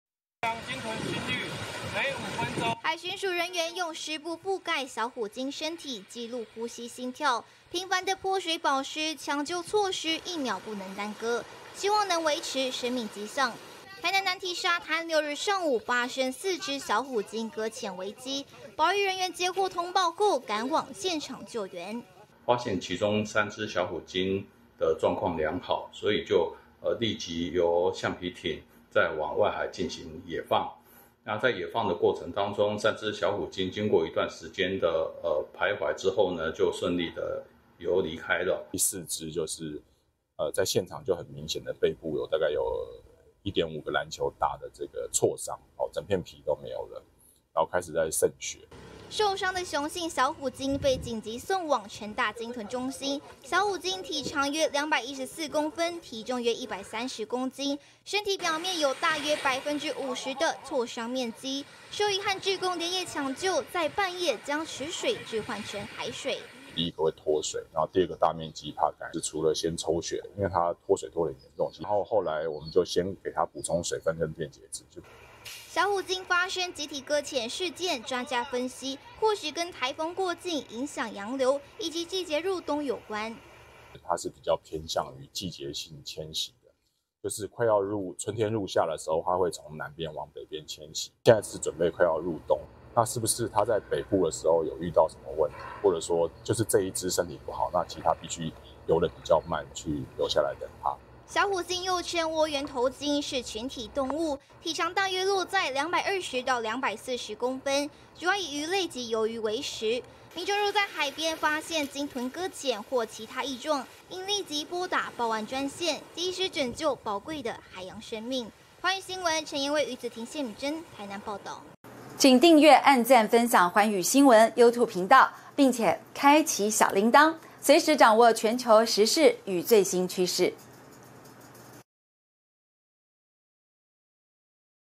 小虎鯨擱淺南堤沙灘「50%挫傷」成大急救治｜寰宇新聞@globalnewstw
台南南堤沙灘6日天發生4隻小虎鯨擱淺事件，經海巡署人員搶救後，3隻小虎鯨已經野放大海，不過有1隻身體挫傷嚴重，緊急送到成大研究中心進行抽血、補水等治療，後續將視復原情況決定野放時間。因為小虎鯨是群體行動的物種，當其中3隻先行野放過程中，還先徘徊好一段時間，疑似就是在等牠們受傷的同伴。